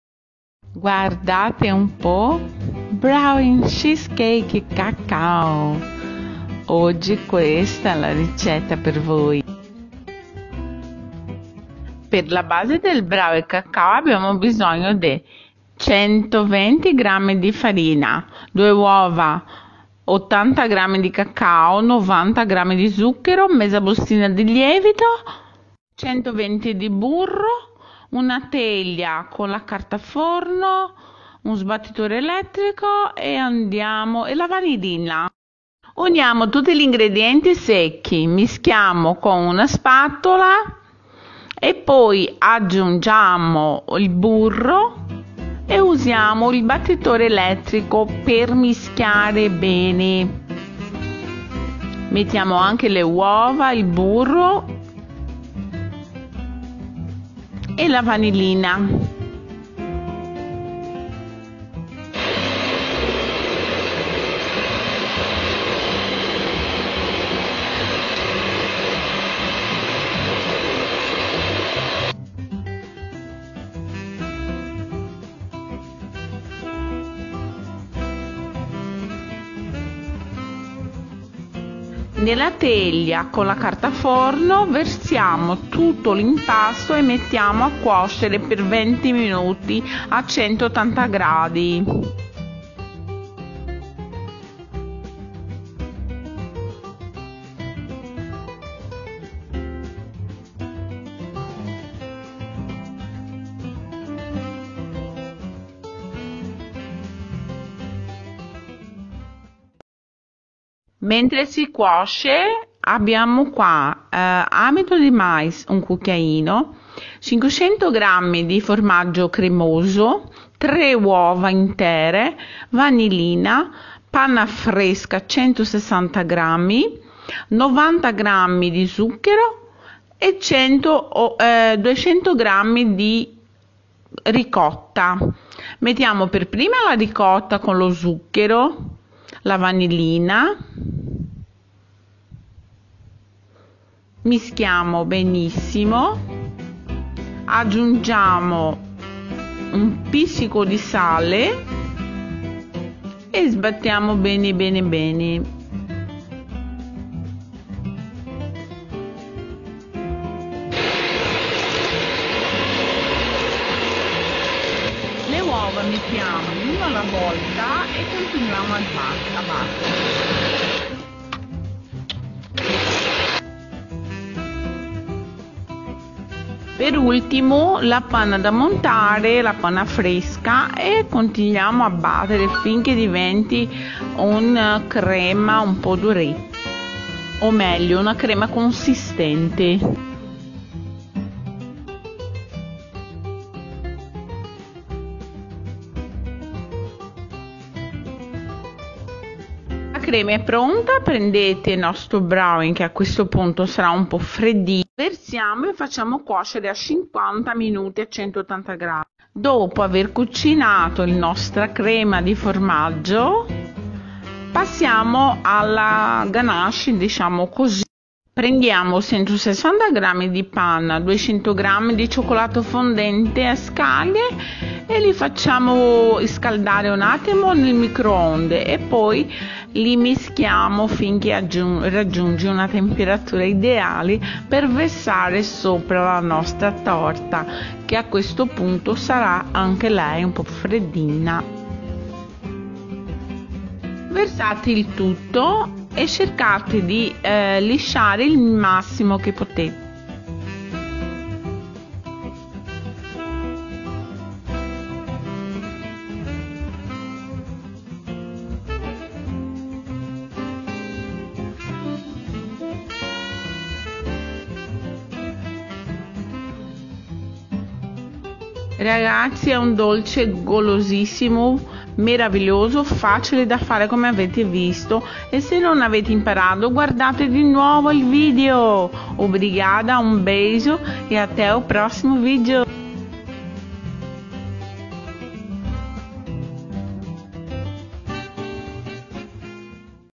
🇮🇪🇮🇪RECEITA EM ITALIANO COM TRADUÇÃO EM PORTUGUÊS
APRENDA ITALIANO COM RECEITAS- BROWNIE CHESSCAKE CHOCOLATE FACIL

BROWNIE CHEESKAKE CHOCOLATE

INGREDIENTES

120 G DE FARINHA
80 G DE CACAU
02 OVOS INTEIROS
90 G DE AçUCAR
120 DE MANTEIGA
BAUNILHA

UNA FORMA FORRADA COM CARTA DA FORNO

PRE AQUECER O FORNO A 180°

UNIMOS TODOS OS INGREDIENTES SECOS E COLOCAMOS OS OSVOS E A MANTEIGA DERRETIDA E A BAUNILHA, BATERE BEM
COLOCAR NA FORMA E ASSAR POR 20 MIN EM FORNO A 180° POR 20 MIN

LA CREMA DI FORMAGGIO

INGREDIENTES

01 COLHER DE  AMIDO DE MILHO
160 G DE CREME DE LEITE FRESCO
500 G DE QUEIJO CREMOSO
03 OVOS INTEIROS
90 G DE A-òUCAR
BAUNILHA
200 G DE RICOTA
BAUNILHA
01 PITADINHA DE SAL

MISTURAR A RICOTA COM O AçUCAR E ACRESENTAR  OS OVOS UM DE CADA VEZ E A BAUNILHA
BATER BEM ATE VIRAR UMA CREMA CONSISTENTE

ACRESCENTAR O CREME DE FORMAGIO NA FORMA E COZINHA EM FORNO QUENTE A 180 ° POR 50 MIN

PER LA COPERTURA DI CHOCOLATE

160 G DE CREME DE CREME DE LEITE MORNO E ACRESCENTAR O CHOCOLATE A PEDACINHOS E MISTURAR BEM

DEIXE ESFRIAR E COLOCAR A COBERTURA DE CHOCOLATE PARA FINHALIZAR E SERVIR
SOBREMESA GULOSA DA JAQUE

DEICE SUGESTOES E SUA OPINIAO